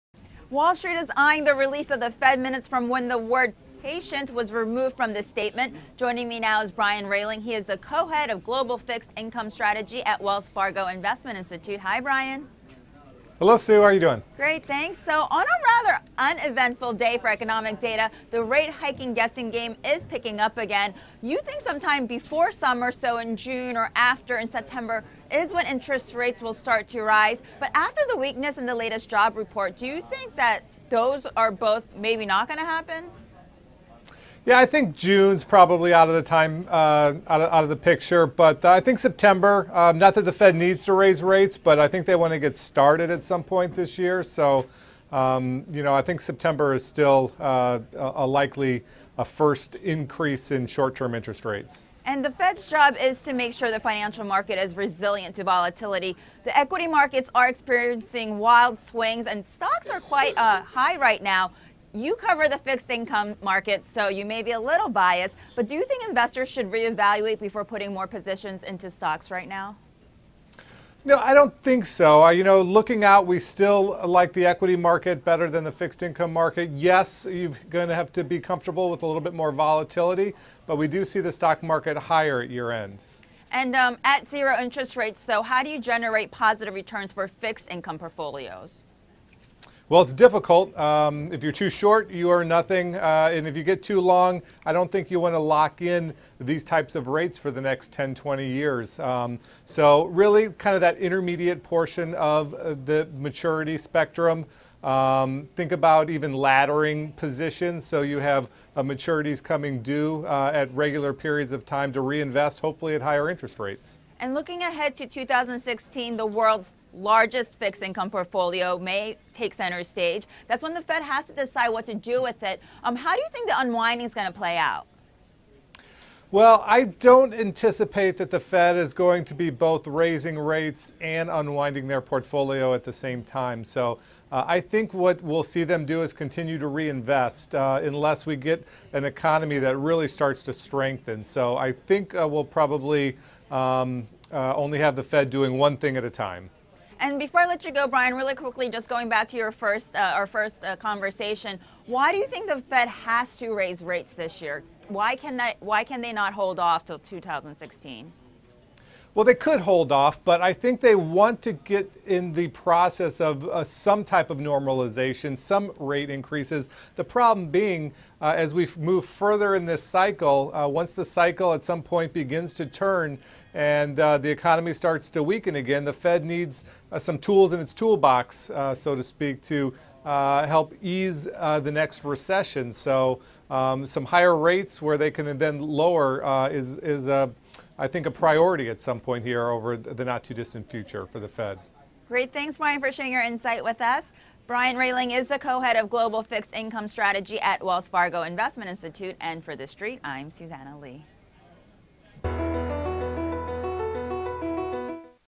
Rate-Hike Guessing Game Picks Up as Investors Wait for FOMC Minutes
The guessing game on when the Fed will raise rates is in full force as Wall Street eyes the release of the Fed minutes from its March meeting when the word 'patient' was removed from the statement. Brian Rehling is the co-Head of Global Fixed Income Strategy at Wells Fargo Investment Institute (WFC) and says despite the weak jobs report, he thinks a hike is still coming in September. Rehling says 'it's not because the Fed needs to raise rates but they want to get started this year.' As for the world's largest fixed-income portfolio, Rehling doesn't see the Fed unwinding in 2016 while raising rates saying 'I think we'll see them continuing to reinvest.'